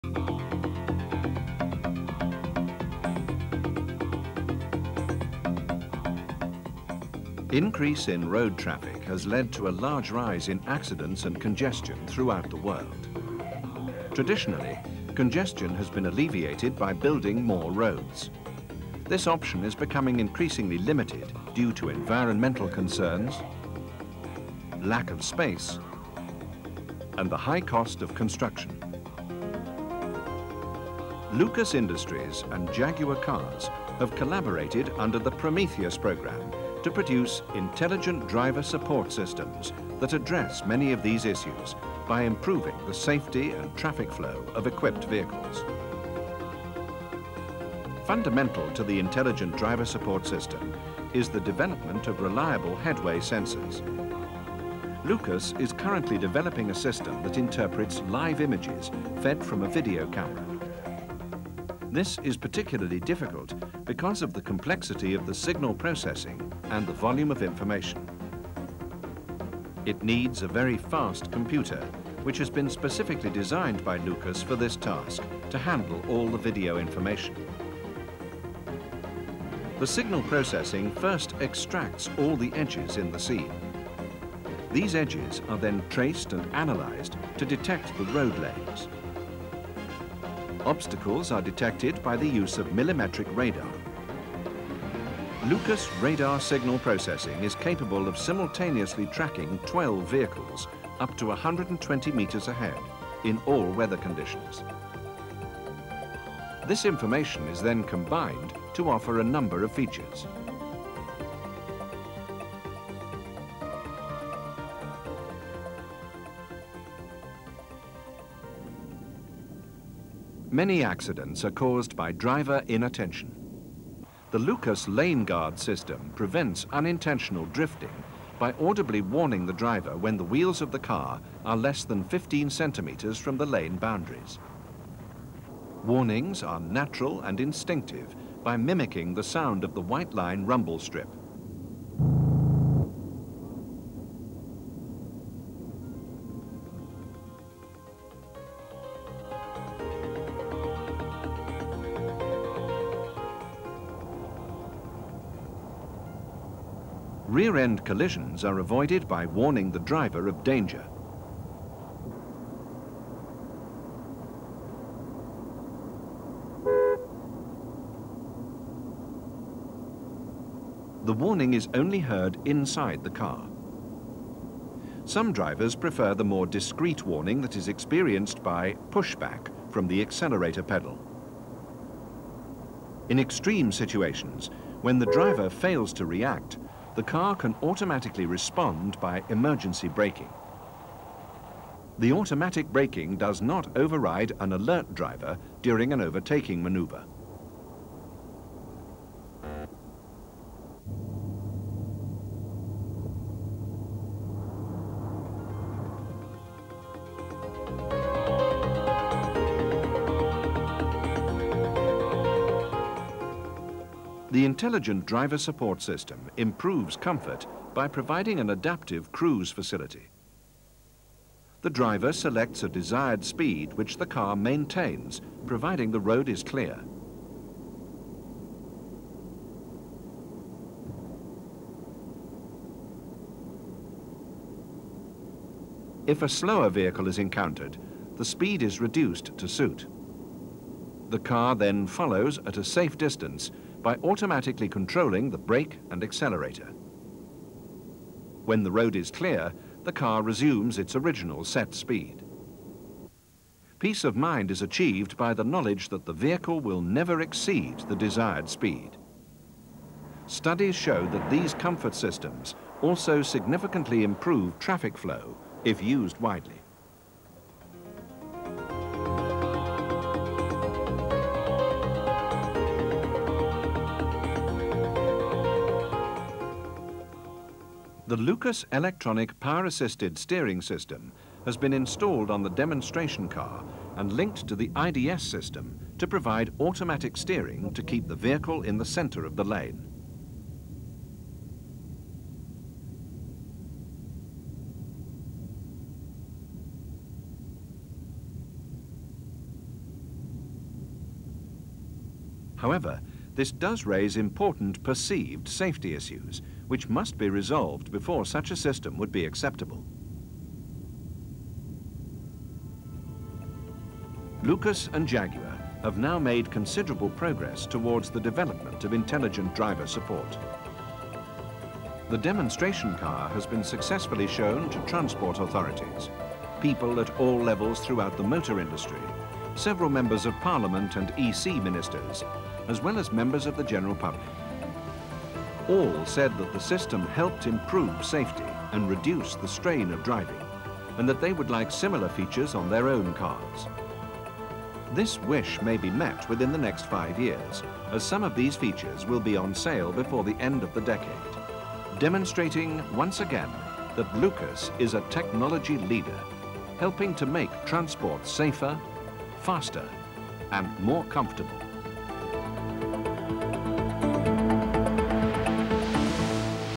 Robot Car | Raglan Tribe
Back in 1994 a team at Lucas Industries developed this autonomous car to showcase adaptive cruise control, collision warning with a haptic throttle, rumble strip warning and automatic lane following.

The team was led by Raglan Tribe and the project was funded by Jaguar Cars, Lucas and the UK Department of Trade and Industry.

The main team members were: Raglan Tribe, Mark Basten, Ian Westwood, Russell Wilson-Jones, Richard Conlong, Ken Maclauchlan, Mansoor Arain, and from Jaguar Cars: Nigel Clark, Alan Hutton and Ian Richardson.

Many of the features are now on the market such as adaptive cruise control and rumble strip warning to help you stay in your lane. However, the most impressive feature, automatic lane following, is still to come.

The project was conducted as part of the European Prometheus programme.

During it's life, the car traveled 17,000 miles autonomously on public highways in the UK, France, Germany and Italy. The car was featured on all the major news networks and was driven by politicians and car company executives throughout Europe.

We got John Hedges from the BBC to do the voice over and the video was made by Gordon Felthouse. I created the script and storyboard.